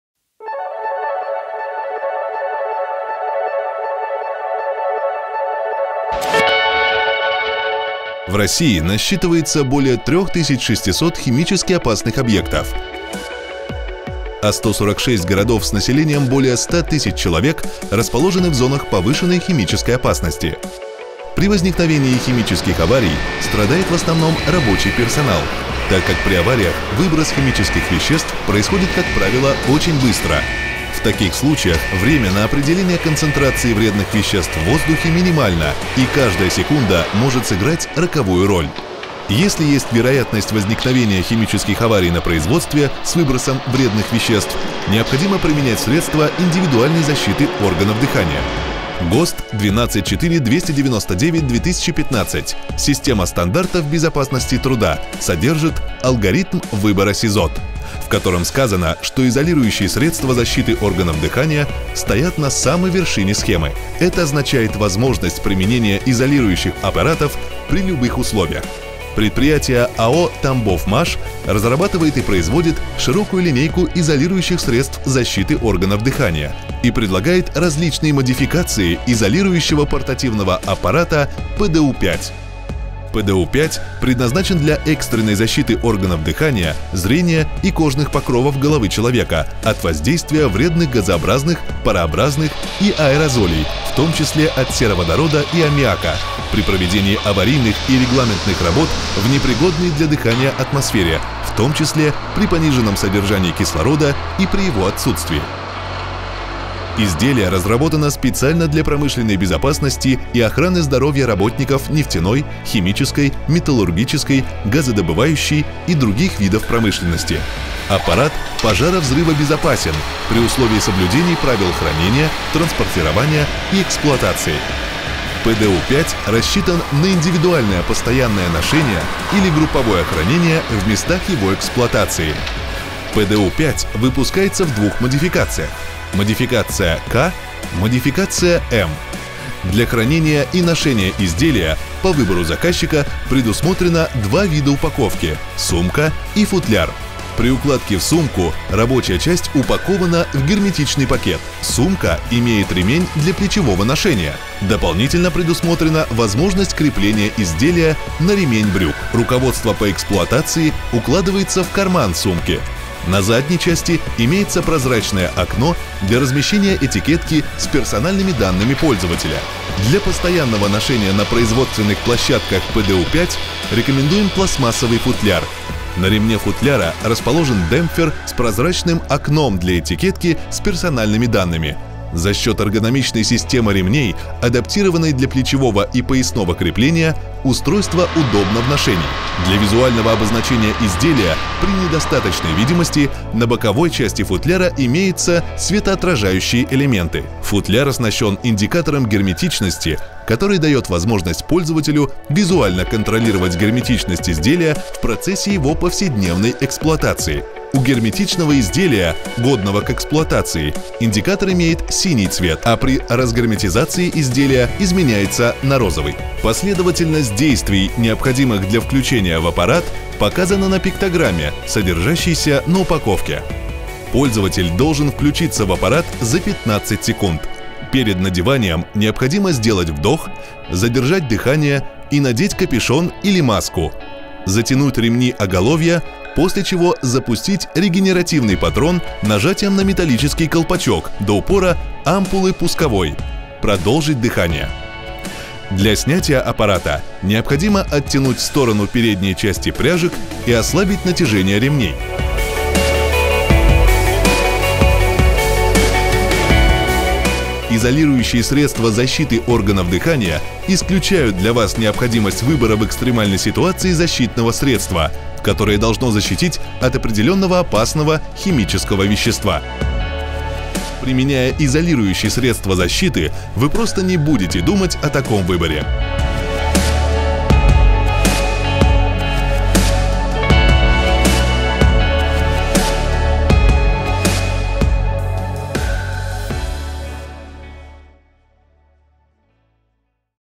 Портативный дыхательный аппарат ПДУ 5
Предназначен для ЭКСТРЕННОЙ защиты органов дыхания, зрения и кожи лица человека в момент возникновения аварий, в период выполнения первичных мероприятий по борьбе с аварией, последующего переключения в табельное средство защиты длительного пользования или эвакуации из аварийного помещения, от любых вредных примесей в воздухе, а также при недостатке кислорода в окружающей среде.